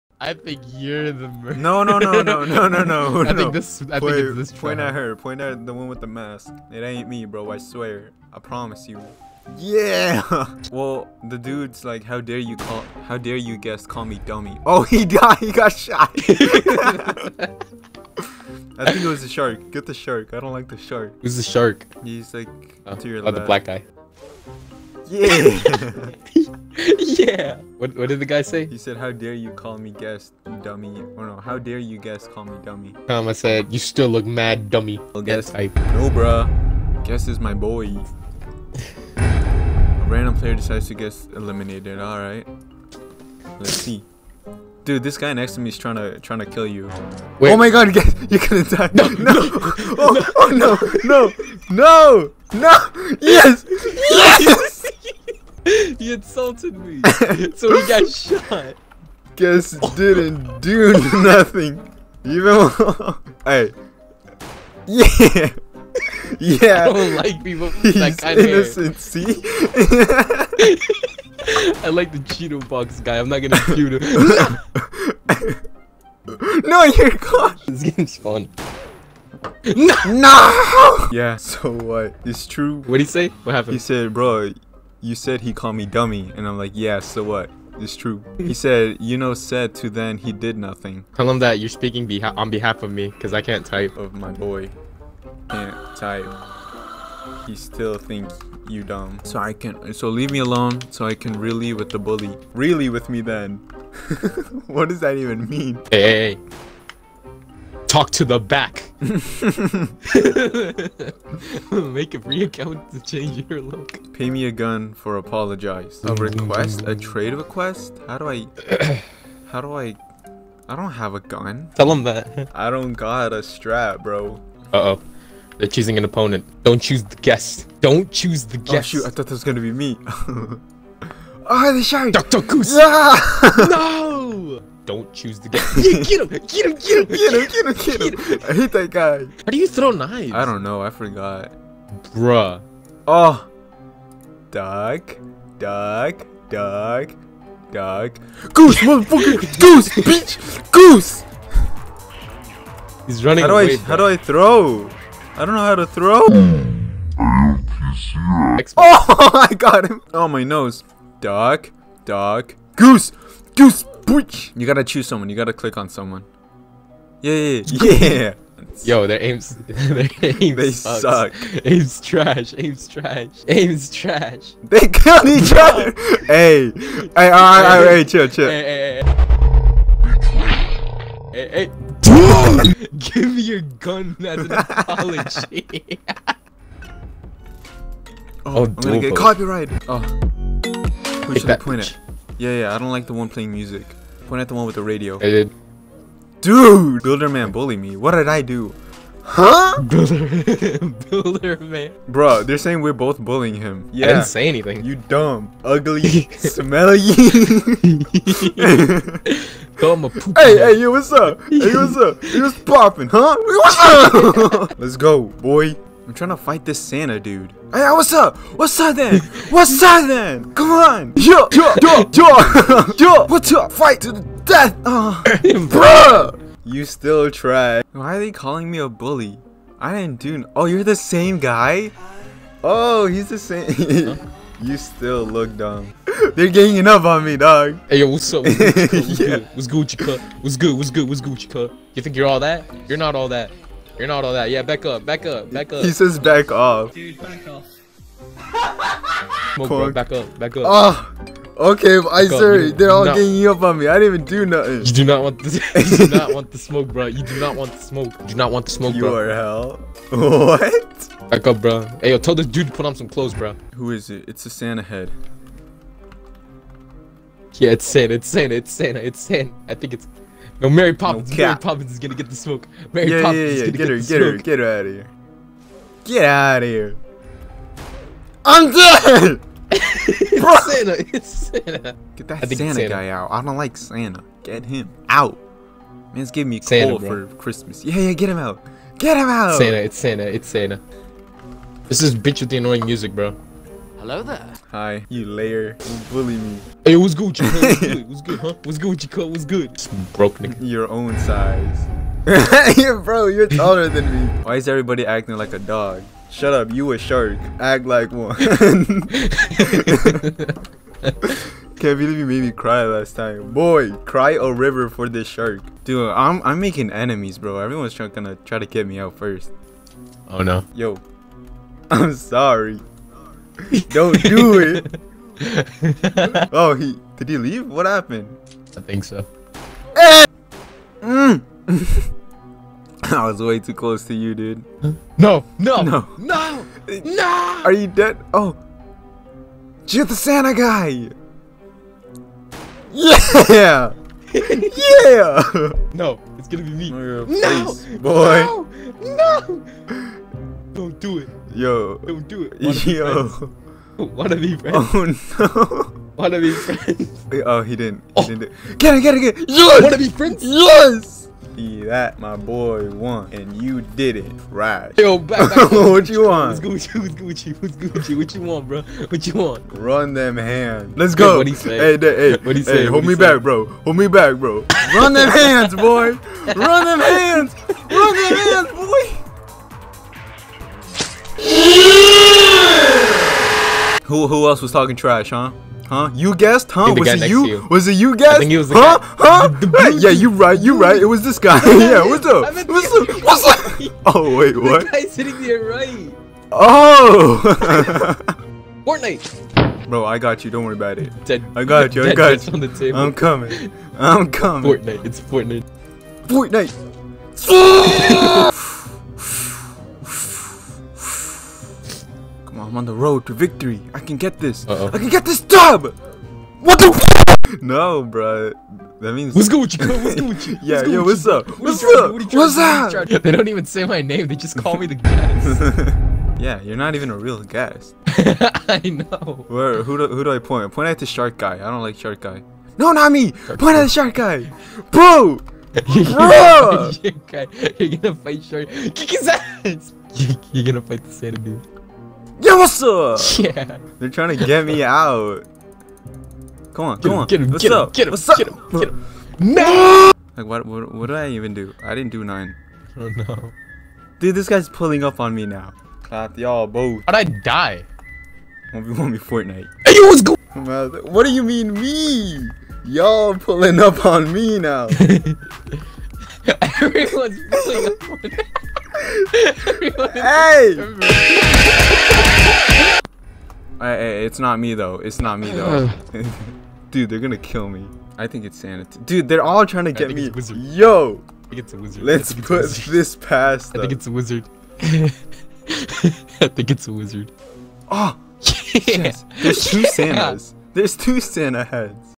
The Entire Lobby HATED Me
all i did was call the dude "dummy dum" cuz my boi thought he looked dum I was just the messenger